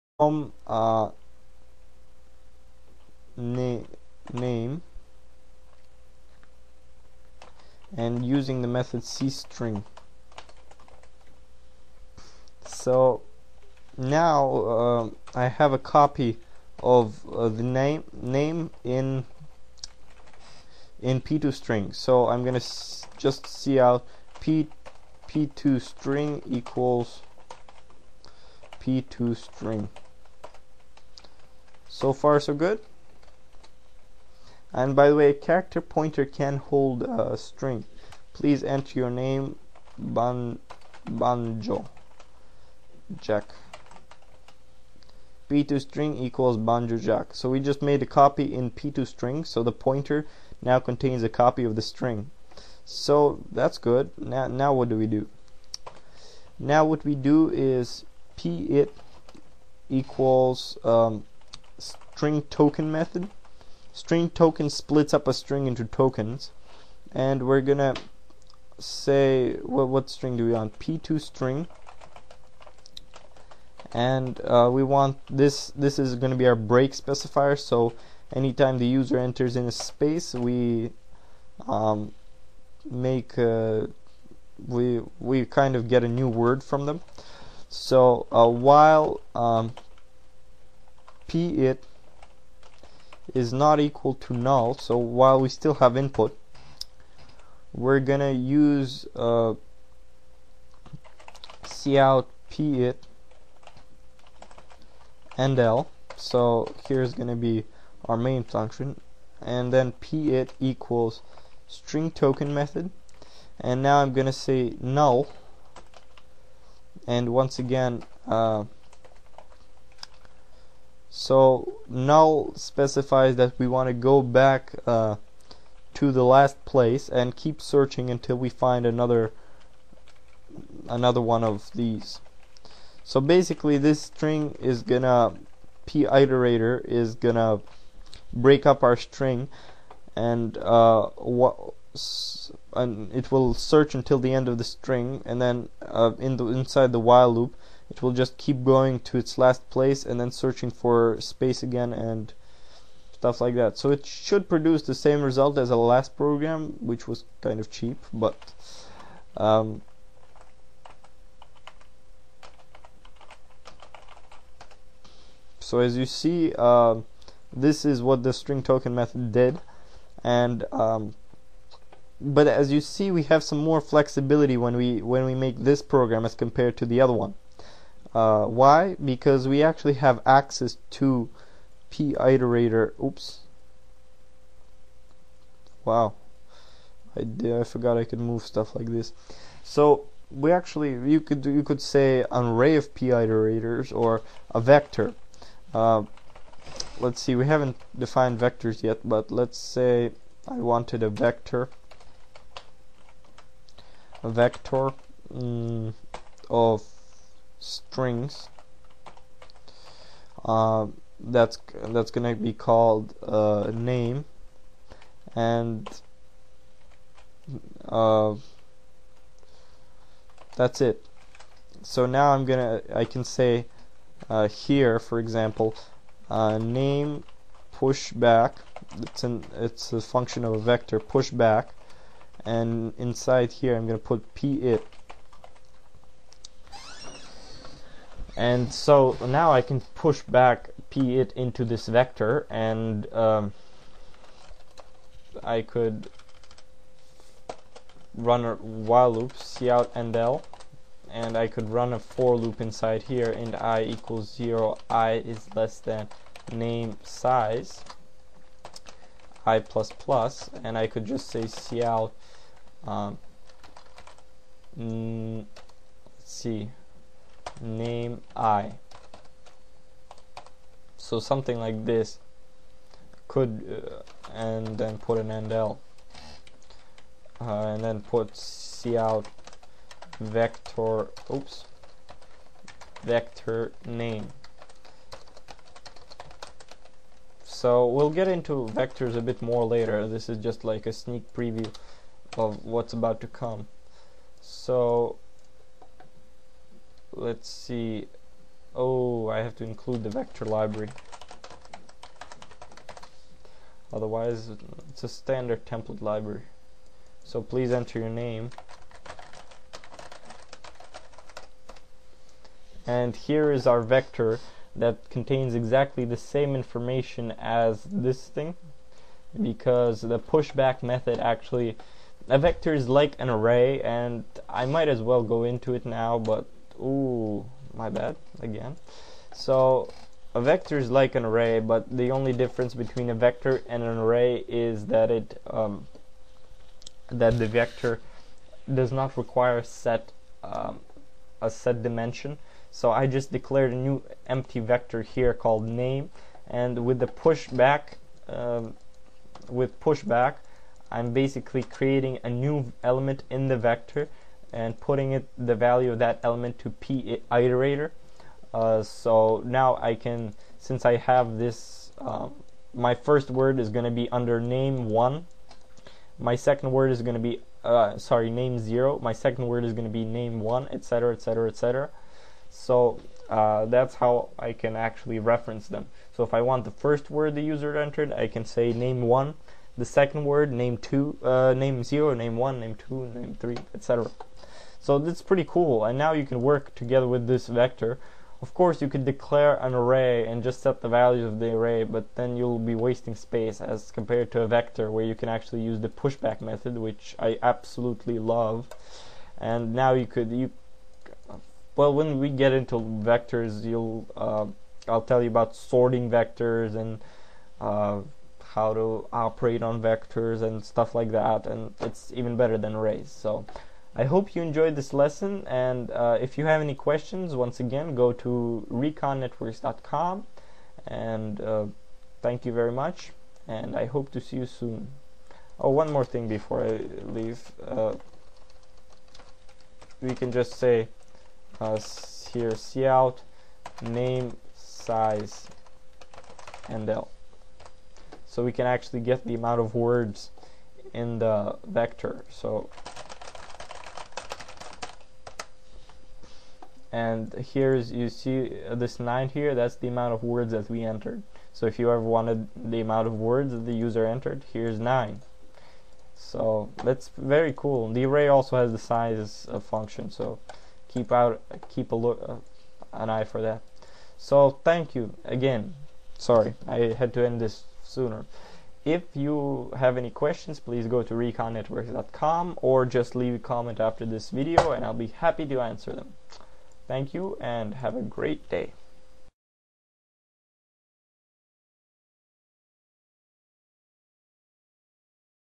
C++ Tutorial #6, Pointers (3)
Pointers - Dynamic Memory Allocations. Use of vectors(not explained, just used)